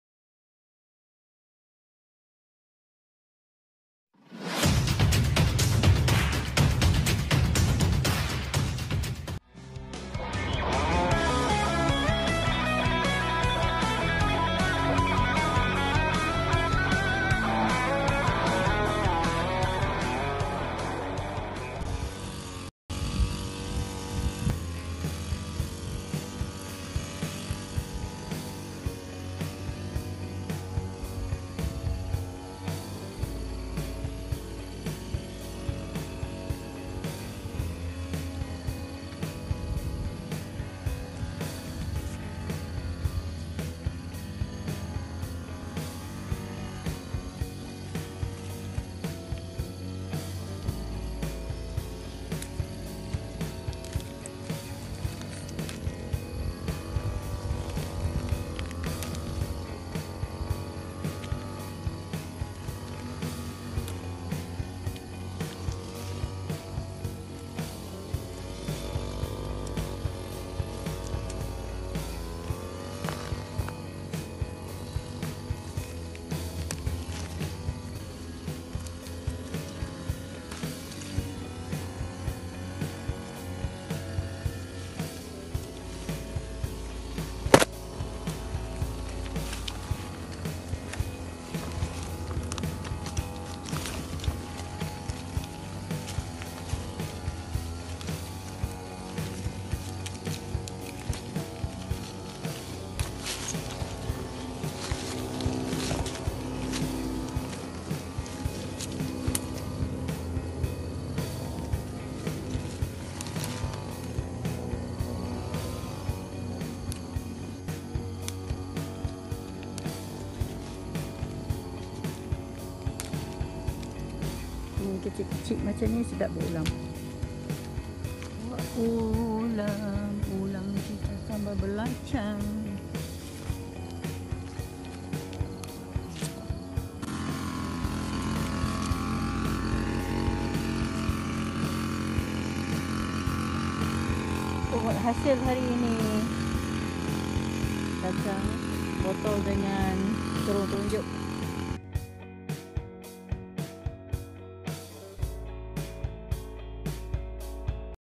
KACANG BOTOL ULAM BERPERISA
Mula tanam.dulu macam.dah takde harapan. Lama sangat dia nak berbuah. Berkat kesabaran dan Lost mortem.. alhamdulillah.
Formula ABC jer.. Air, Baja dan Cahaya Matahari.  Rupa ya dia lambat berbuah sebab ternaung dan kurang mendapat cahaya matahari.
Tapi sekarang.. setiap minggu kalau datang mesti dia bagi bekal buat ulam ketika makan bersama keluarga.  Alhamdulillah..  semua rezeki tu datang dari Allah SWT.

Alhamdulillah..KOBUN DEN berkembang dengan baik. Harap aktiviti yang berterusan dapat berkekalan.